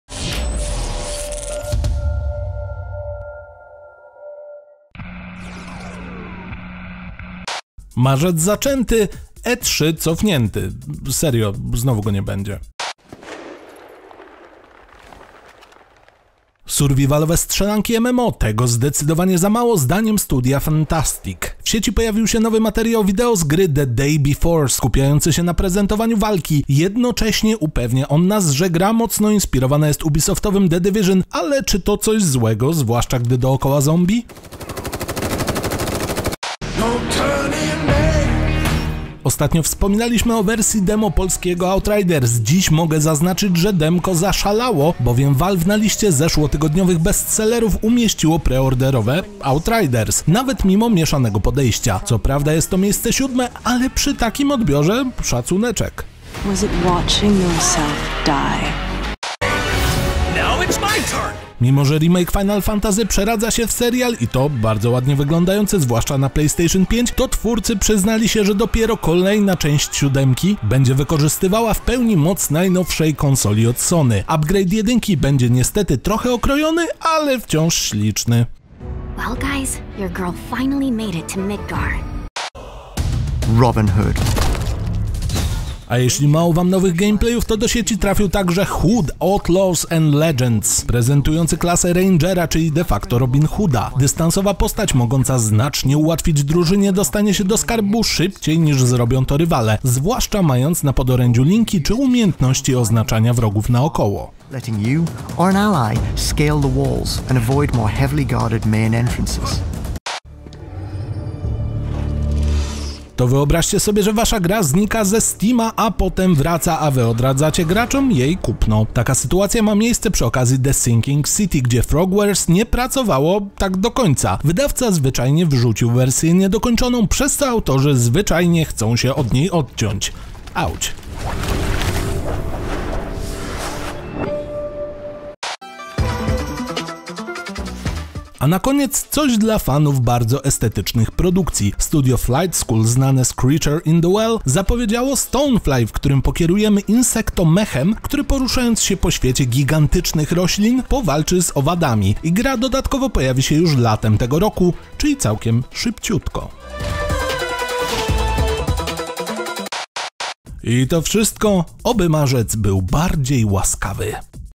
Twórcy tej gry odradzają zakup na Steam. FLESZ – 1 marca 2021
Twórcy The Sinking City odradzają zakup gry na Steam, bo ta wersja jest podobno niedokończona. We Fleszu mówimy też o rozgrywce z The Day Before i Hood: Outlaws & Legends a także zapowiedzi gry Stonefly.